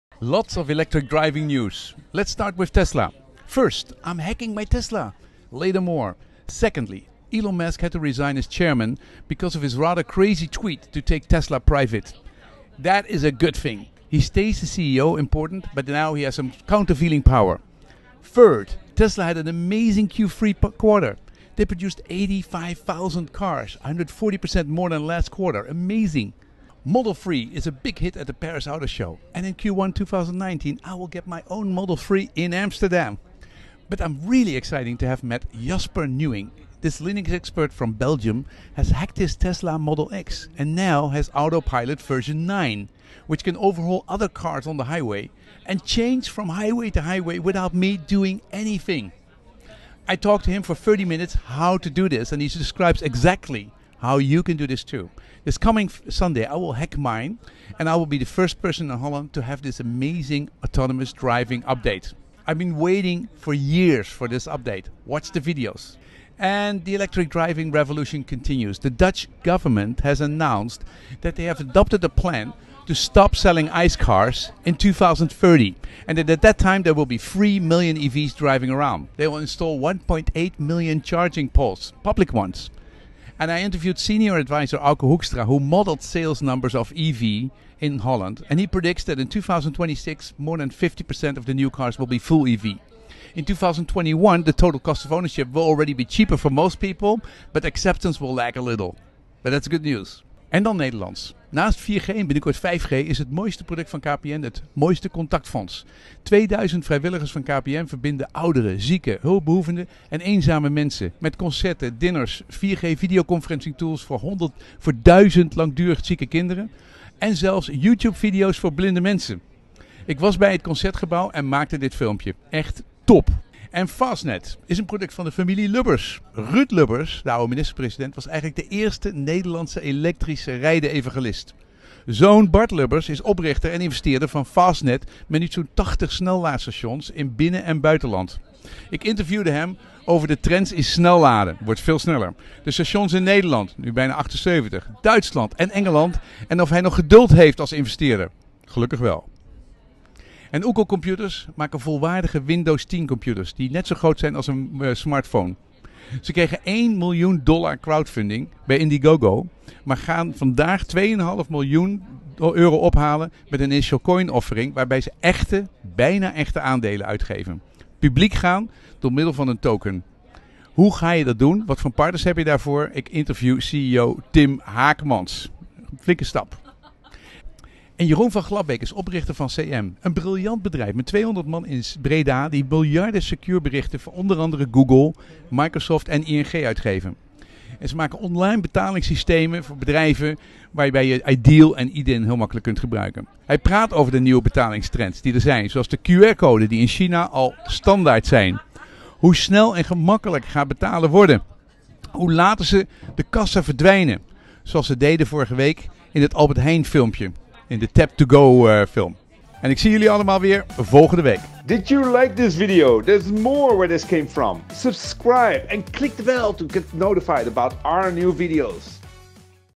Hacking my TESLA MODEL X! Elon Musk, EV in NL, FASTNED, KPN Mooiste Contact Fonds & Payment Trends
Lots of electric driving news!

Lets start with Tesla! Elon Musk had to resign as chairman because of his rather crazy tweet to take Tesla private. That is a good thing. He keeps being the CEO, important, but now has some counter-veiling power. Tesla had an amazing Q3 quarter. They Produced 85.000 cars. That's 140% more than last quarter. Amazing. Model 3 is a big hit at the Paris AutoShow and in Q1 2019 I will get my own Model 3 in Amsterdam.

I'm hacking my Tesla! Jasper Nuyens, founder of Linux Belgium, has hacked his Tesla Model X and now has the new Autopilot Version 9. How does it work and what can it do? An in-depth 30 minutes interview. This Sunday he updates my Model X as well and I will be the the first person in Holland to have this amazing autonomous driving update!

The Dutch government has announced that have adapted a plan to stop selling ICE cars in 2030 and at that time there will be 3 million EVs driving around. They will also install 1.8 million charging polls. How long will it REALLY take for 50% of all car sales to be electric in The Netherlands? Auke Hoekstra shows the models: Theory vs Reality!

 Now Dutch: het KPN Mooiste Contact Fonds organiseerden een middag voor ouderen tijdens de Week Tegen De Eenzaamheid. Een interview met directeur Mark Versteegen over de nieuwe projecten om mensen te verbinden en prachtige reacties van de deelnemers en vrijwilligers.

FASTNED heeft 80 snellaadstations in Nederland. Oprichter Bart Lubbers over nieuwe stations, de staat van EV in NL, Tesla, Shell en laden in 5 minuten!

Nederlandse startup Ockel Computers haalde al $1 miljoen op met crowdfunding op Indiegogo en gaat nu via een Security Token ICO $2.5 miljoen ophalen. Hoe werkt dat? Een interview met CEO Tim Haaksma.

En een interview met Jeroen van Glabbeek, oprichter van CM.com - SMS & Payment provider die AH Tap To Go mogelijk heeft maakt. Welke trends ziet hij in het betalingsverkeer wereldwijd?